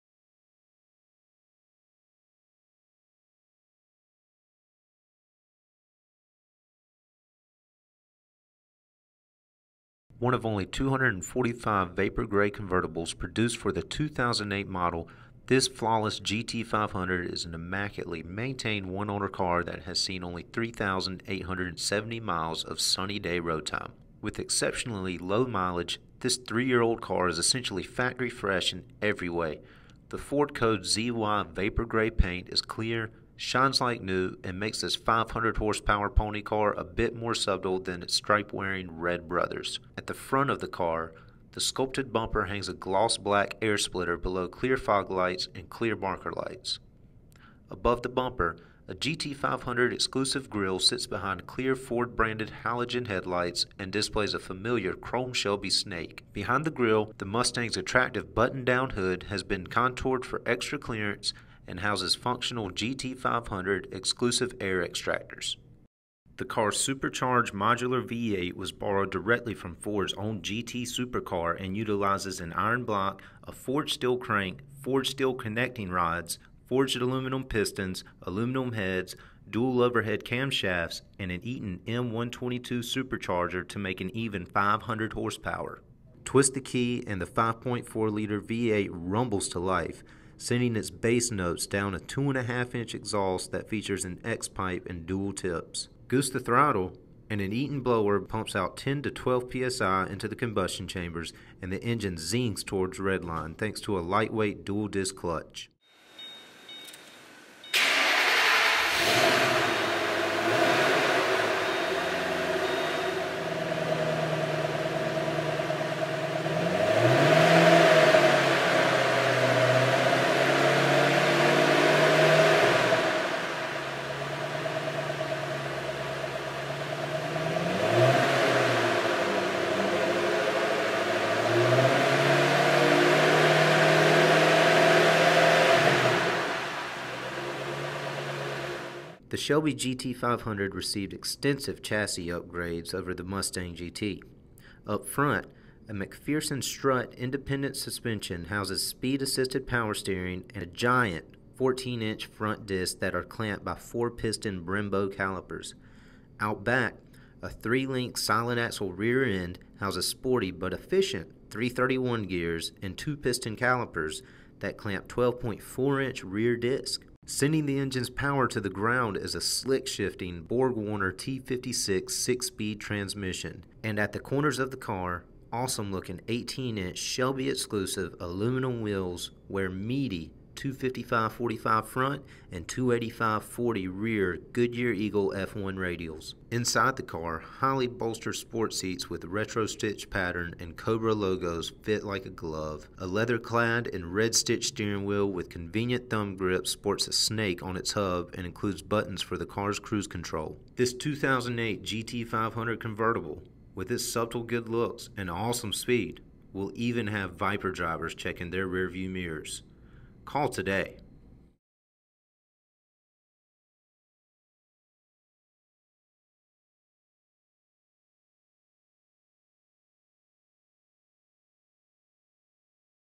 132621 / 2008 Ford Mustang GT500
One of only 245 Vapor Gray convertibles produced for the 2008 model, this flawless GT500 is an immaculately maintained one owner car that has been seen only 3,870 miles of sunny day road time.

With exceptionally low mileage, this three year old car is essentially factory fresh in every way. The Ford code ZY Vapor Gray paint is clear, shines like new and makes this 500 horsepower pony car a bit more subtle than its stripe-wearing red brothers. At the front of the car, the sculpted bumper hangs a gloss black air splitter below clear fog lights and clear marker lights. Above the bumper, a GT500 exclusive grille sits between clear Ford branded halogen headlights and displays a familiar chrome Shelby snake. Behind the grille, the Mustangs attractive buttoned down hood has been contoured for extra clearance and houses functional GT500 exclusive air extractors. At the sides of the car, clean black rocker trim complements sculpted black mirrors and black GT500 door stripes that run from the front wheels to the rear wheels. Above the GT500 stripes, two more chrome Cobra emblems sit just behind the front tires in the center of the front fenders. At the back of the car, an aggressive rear decklid spoiler sits above clear segmented Mustang tail lights and a silver and black "Shelby" emblem that's mounted on the driver's side of the trunk. Below the trunk, a tapered rear bumper hangs a black air splitter above stainless exhaust tips.

The 500 horsepower supercharged V8 was influenced heavily by Fords own GT super car and utilizes an iron block, a forged steel crank, forged steel connecting rods, forged aluminum pistons, aluminum heads, dual overhead camshafts and an Eaton M122 supercharger to make an even 500 horsepower. Twist the key and the 5.4-liter V8 rumbles to life, sending its bass notes down a 2.5-inch exhaust that features an X-pipe and dual tips. Goose the throttle and an Eaton blower pumps 10 to 12 PSI into the combustion chambers, and the engine zings towards redline thanks to a lightweight dual disc clutch. The engine bay looks very clean, not featuring any of the flash you'd expect for such a pedigreed car and appearing, dare I say, a bit like a sleeper. But to anyone who knows anything about cars in general, the satin black blower at the top of the block, the small silver SVT badge and the silver "Powered by SVT" cam covers are a dead giveaway that they're looking at something special.

The Shelby GT500 received extensive chassis upgrades over the base Mustang GT. Up front; a Macpherson strut independent suspension houses speed assisted power steering and giant 14 inch front discs that are clamped by 4 piston Brembo calipers. Out back; a three link solid axle rear end houses sporty but efficient 3.31 gears and 2 piston calipers that clamp 12.4 inch rear discs. Sending the engines power to the ground is a slick shifting Borg Warner T56 6-speed transmission. And at the corners of the car, awesome looking 18 inch Shelby exclusive aluminum wheels wear meaty 255/45 front and 285/40 rear Goodyear Eagle F1 radials.

Inside the car, highly bolstered sport seats with a retro stitch pattern and Cobra logos fit like a glove. A leather-clad and red stitched steering wheel with convenient thumb grips sports a snake on its hub and includes buttons for the cars cruise control. Behind the steering wheel, the silver trimmed instrument panel is stock Mustang, and features Fords infinitely changeable dash lighting. The graphite faces with white lettering have a small SVT logo that serves as a programmable shift light which along with aluminum accented pedals and an aluminum accented and red stitched shifter tips you off to the car's pedigree. The dash around those instruments is some of Fords best modern work featuring a heritage inspired dual cowl design with gray trim with silver accents. At the center of the dash, Fords Shaker sound system features an AM/FM radio, a six disc CD changer, Sirius satellite radio and MP3 compatibility above knobs for the cars climate control.

This 2008 GT500 convertible, with its subtle good looks and awesome speed, will have even Viper drivers checking their rear-view mirrors.